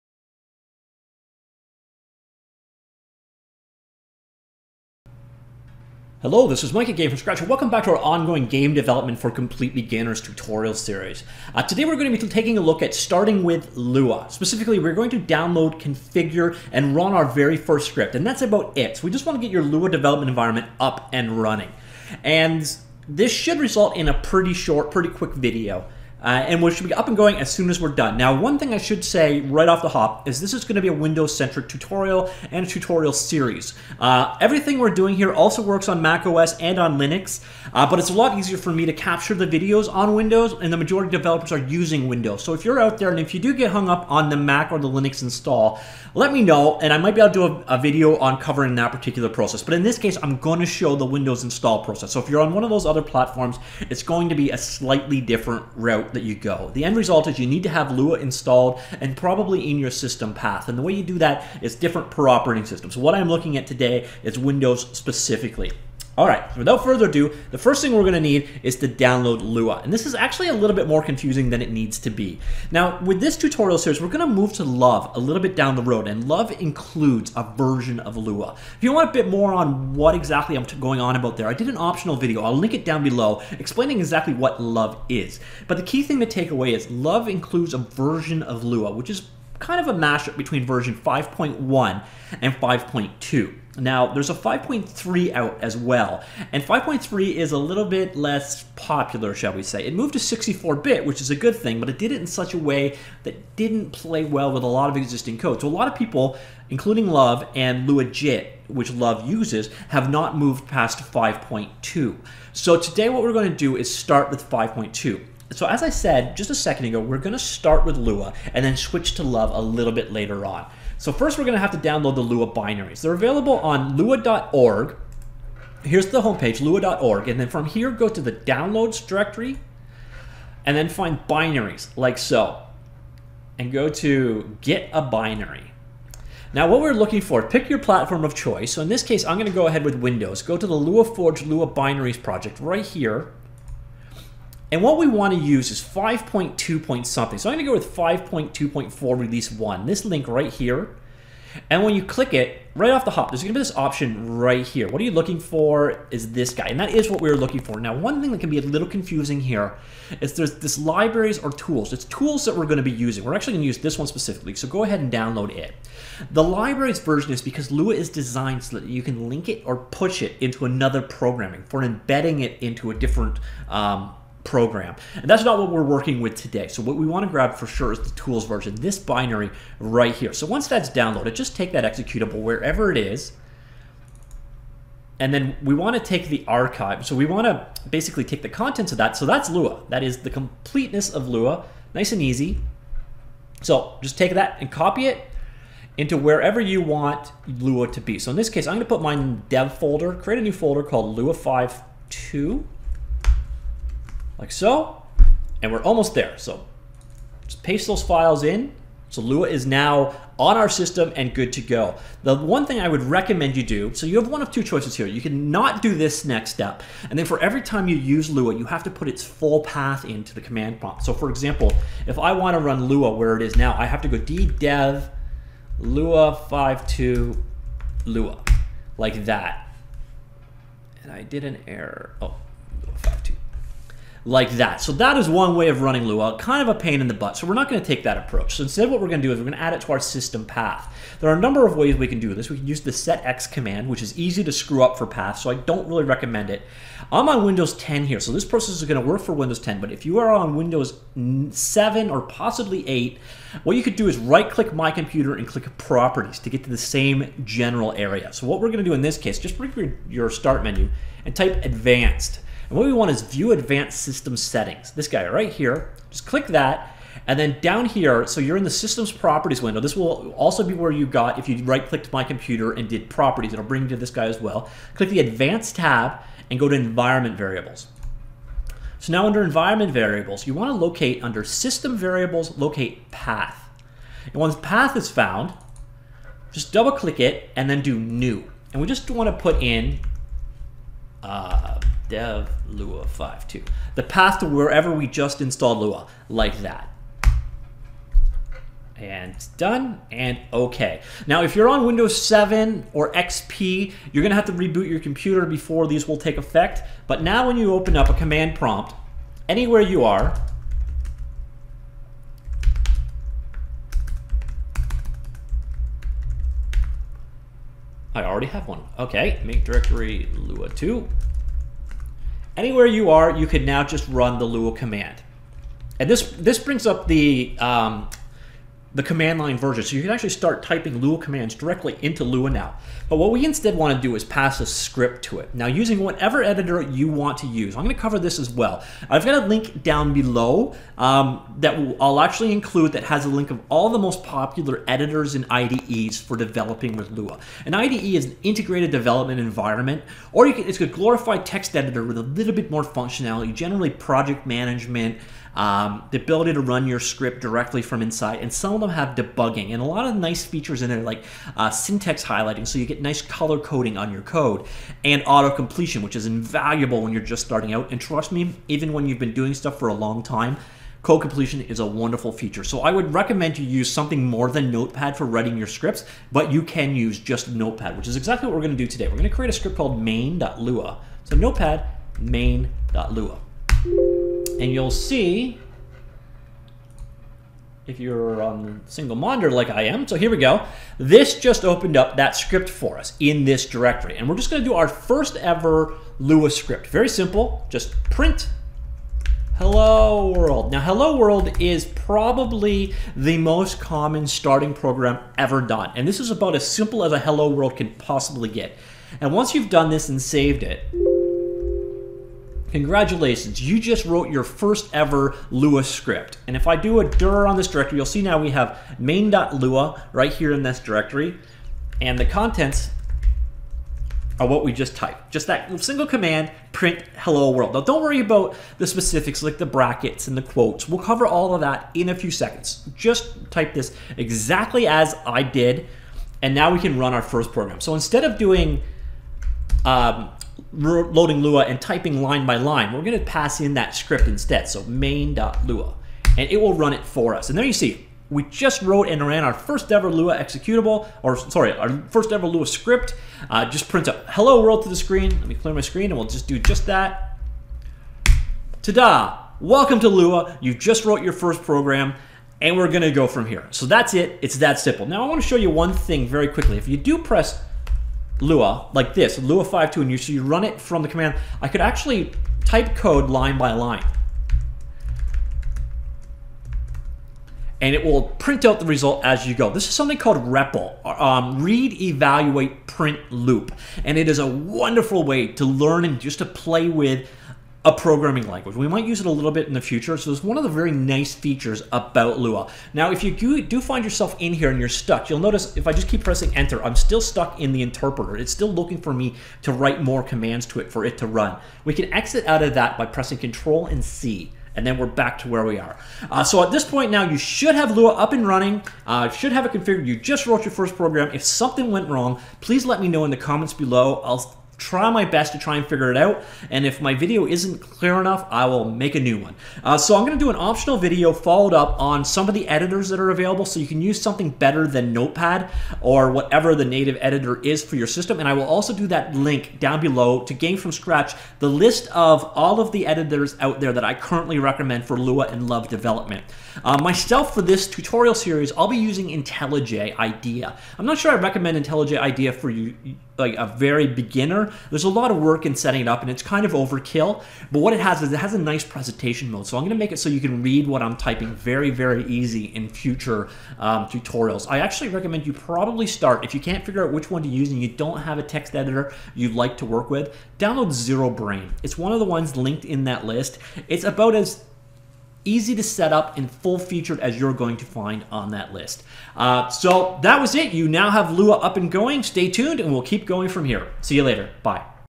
Tutorial 1: Getting Started With Lua -- GameDev for Beginners Series
In this tutorial we get things underway by installing and configuring Lua, creating and running our very first extremely simple program.  By the end of this tutorial Lua should be up and running on your computer.

Links mention in this tutorial ---

Video giving more info on what exactly love is and what it's relationship to Lua is: